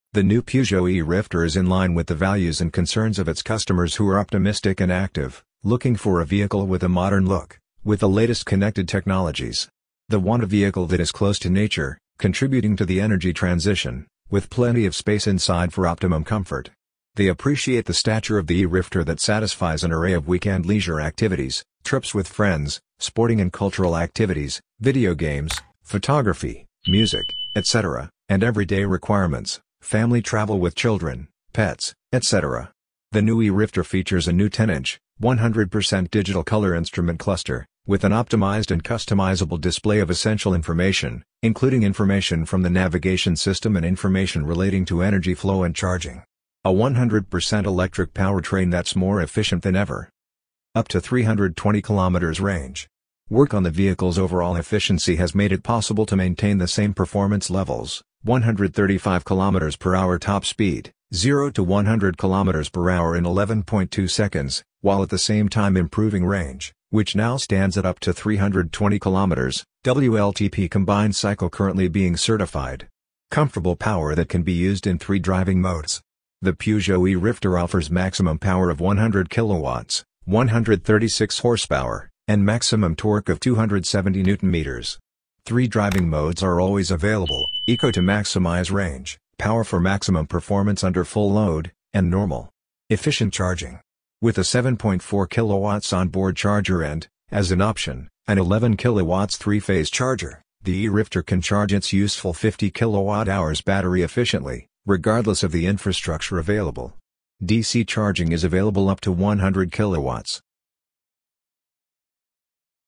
NEW Peugeot E-RIFTER Facelift 2024 | FIRST LOOK
The new PEUGEOT E-RIFTER introduces the brand’s new design language, retaining the muscular, solid and friendly style that underpins its strong adventurous character.It has an electric range that now reaches 320km.

Source:Peugeot